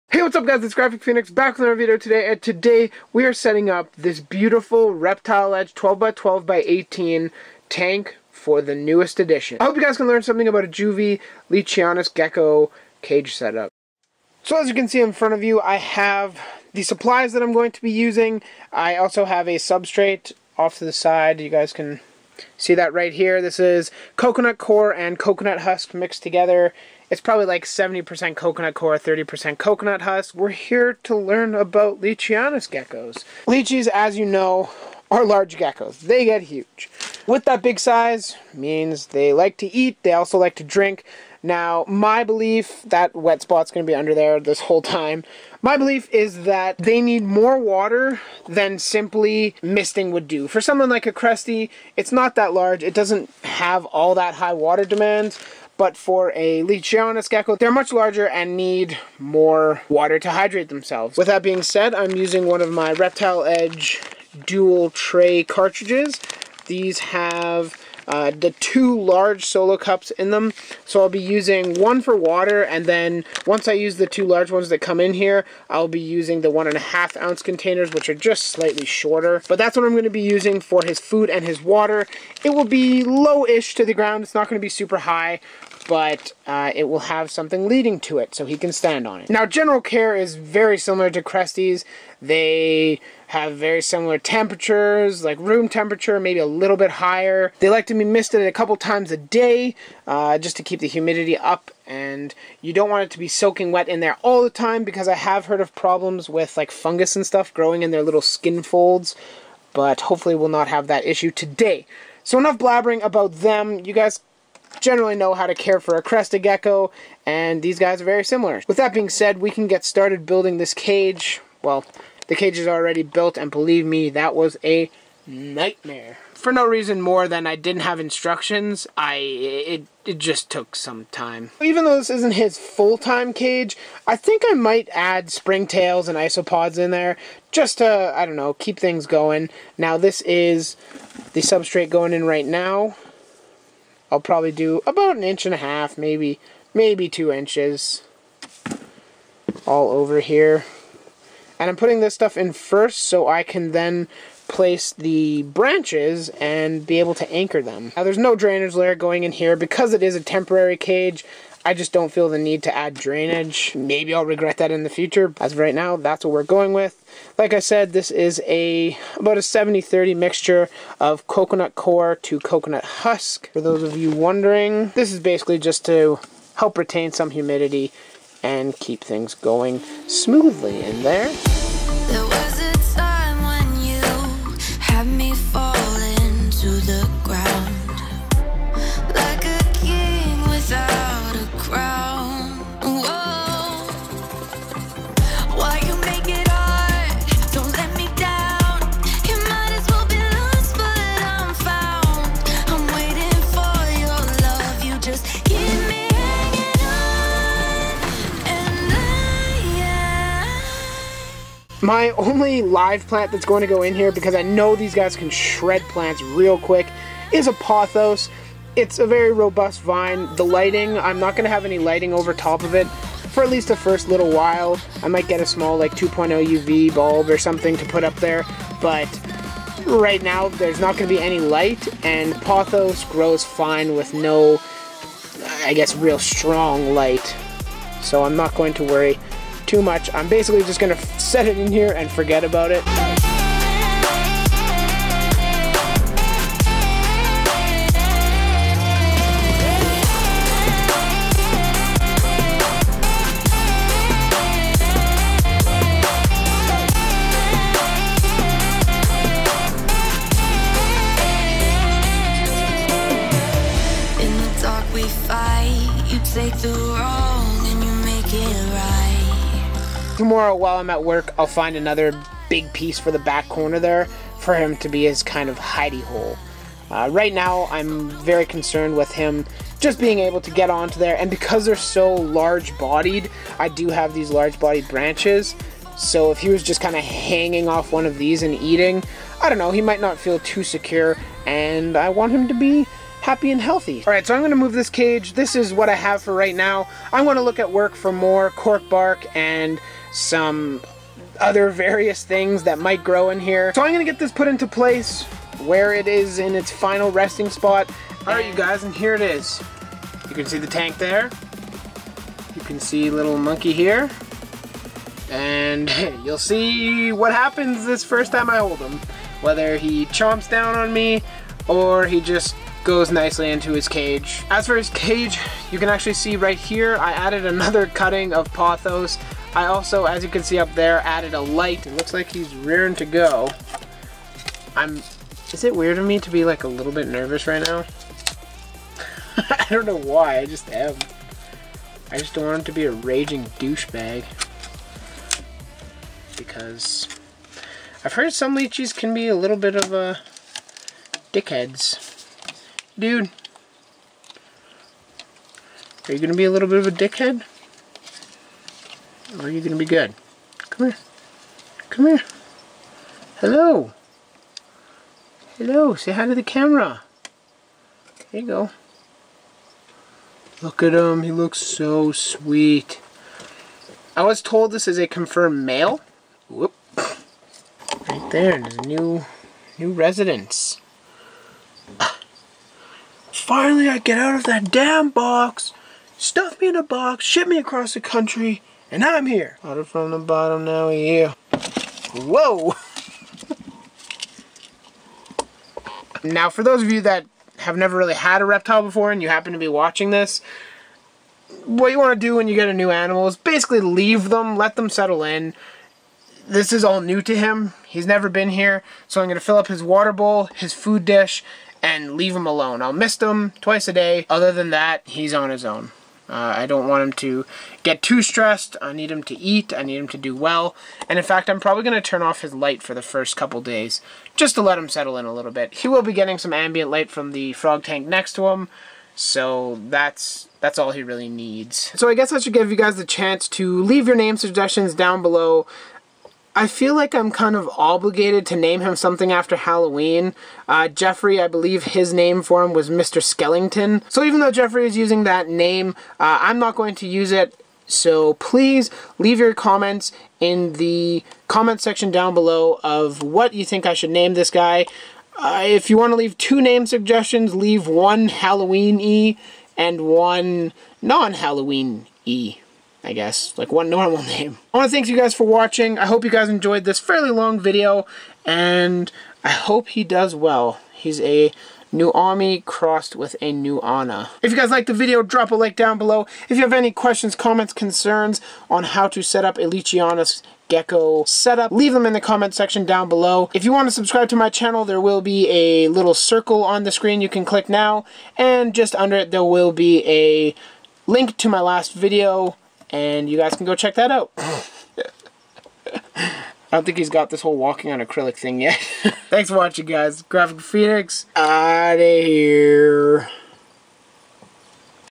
Leachianus Gecko Setup: How to!
leachianus gecko setup for all of you that might be looking to get one or are just curious how cute he is! Leachies are awesome everyone, I hope you have a great weekend!